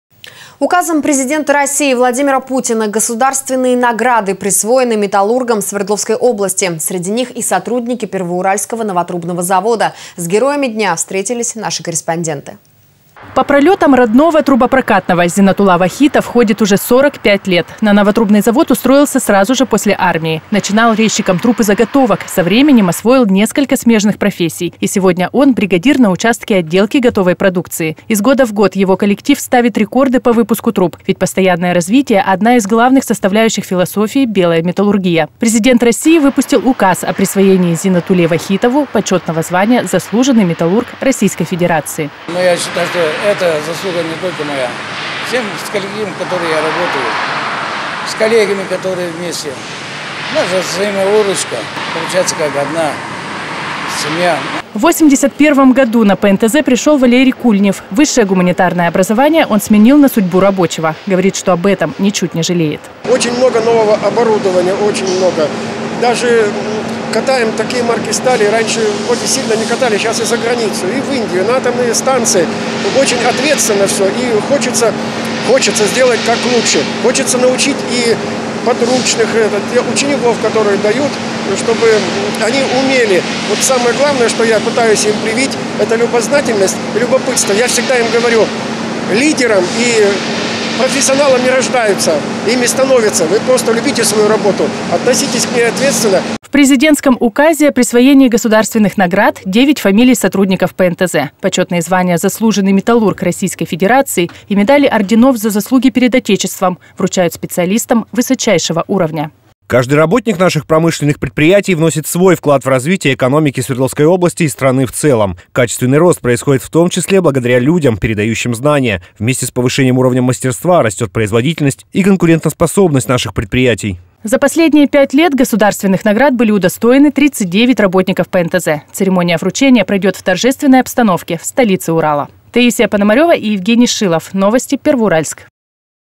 Первоуральские металлурги получили государственные награды
Указом Президента России Владимира Путина государственные награды присвоены металлургам Свердловской области. Среди них и сотрудники Первоуральского новотрубного завода. С героями дня встретились наши корреспонденты.

По пролетам родного трубопрокатного Зинатулла Вахитов ходит уже 45 лет. На Новотрубный завод устроился сразу же после армии. Начинал резчиком труб и заготовок, со временем освоил несколько смежных профессий, и сегодня он - бригадир на участке отделки готовой продукции. Из года в год его коллектив ставит рекорды по выпуску труб. Ведь постоянное развитие – одна из главных составляющих философии Белая металлургия. Президент России выпустил Указ о присвоении Зинатулле Вахитову почетного звания "Заслуженный металлург Российской Федерации".

Зинатулла ВАХИТОВ, Заслуженный металлург РФ, бригадир трубопрокатного цеха ПНТЗ: «Я считаю, что это заслуга не только моя - всего коллектива, с которым я работаю, с коллегами которыми вместе. У нас же взаимовыручка, друг другу помогаем. Получается, как одна семья».

В 81-ом году на ПНТЗ пришел Валерий Кульнев. Высшее гуманитарное образование он сменил на судьбу рабочего. Говорит, что об этом ничуть не жалеет.

Валерий КУЛЬНЕВ, Заслуженный металлург РФ, вальцовщик трубоволочильного цеха ПНТЗ: «Очень много нового оборудования, очень много. Катаем такие марки стали, раньше действительно не катали, сейчас и за границу, и в Индию, на атомные станции. Очень ответственно всё и хочется сделать, как лучше. Хочется научить и учеников, чтобы они умели. Самое главное, что я пытаюсь им привить - это любознательность и любопытство. Я всегда им говорю - лидером и профессионалом не рождаются, им становятся. Вы просто любите свою работу, относитесь к ней ответственно».

В президентском Указе о присвоении государственных наград - 9 фамилий сотрудников ПНТЗ. Почетные звания "Заслуженный металлург Российской Федерации" и медали орденов "За заслуги перед Отечеством" вручают специалистам высочайшего уровня.

Игорь ЗЕЛЕНКИН, и.о. министра промышленности и науки Свердловской области: «Каждый работник наших промышленных предприятий вносит свой вклад в развитие экономики Свердловской области и страны в целом. Качественный рост происходит, в том числе, благодаря людям, передающим знания. Вместе с повышением уровня мастерства, растет производительность и конкурентоспособность наших предприятий».

За последние пять лет государственных наград были удостоены 39 работников ПНТЗ. Церемония вручения пройдет в торжественной обстановке, в столице Урала.